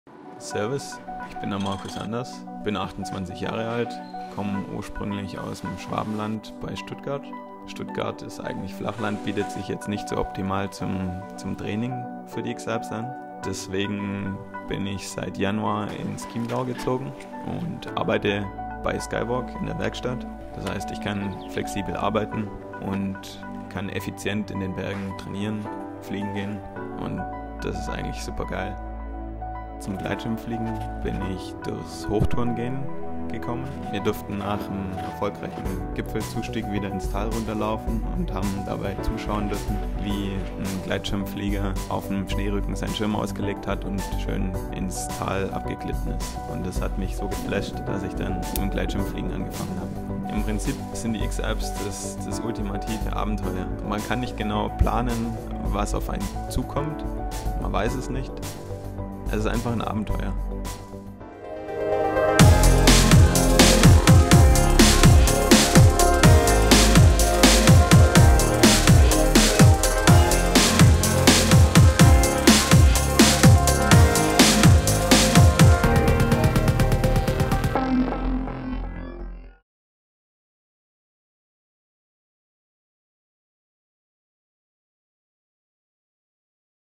skywalk paragliders | X-Alps Athlete Portrait: Markus Anders
We spent the day with our Red Bull X-Alps athlete Markus Anders (Team GER2). Markus is originally from Stuttgart but moved to the Chiemgau in January to work for skywalk and train for the Red Bull X-Alps 2019.

We hiked up to the Kampendwand, which is one of this years turnpoints in the event, and had the chance to ask a few questions.

Next portrait will be: Simon Oberrauner